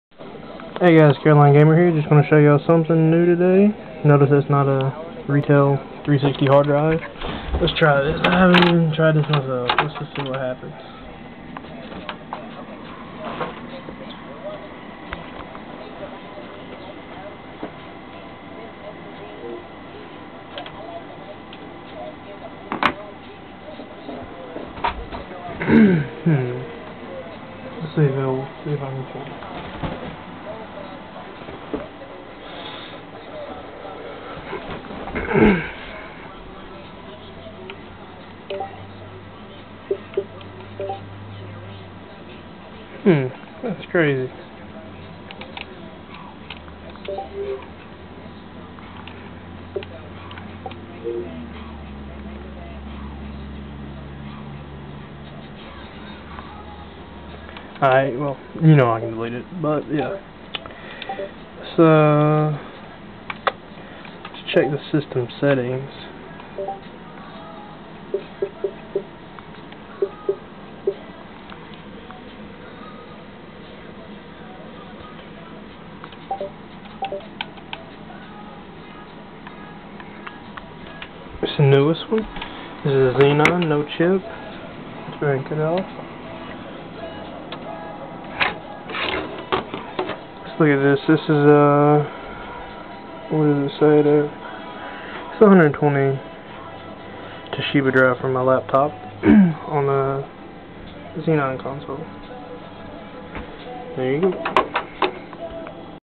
XBOX 360 Single NAND Rebooter XBR
This uses no chip to reboot the console from a single nand images, this video just show a normal HDD from my laptop connected to it. I'll have more videos up later on. This is a Xenon Console.